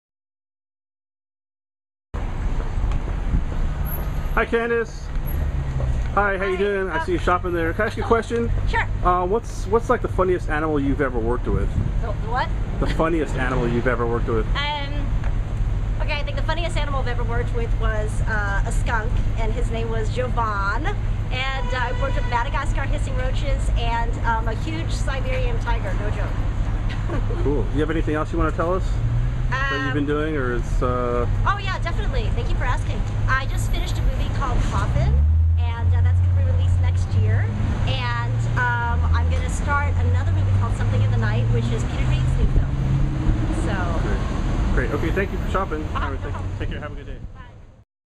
Candace Kita Talks About The Funniest Animals On CelebrityPopCulture.com
The beautiful and talented Candace Kita  Talks About  The Funniest Animals  she's ever worked with On CelebrityPuke.com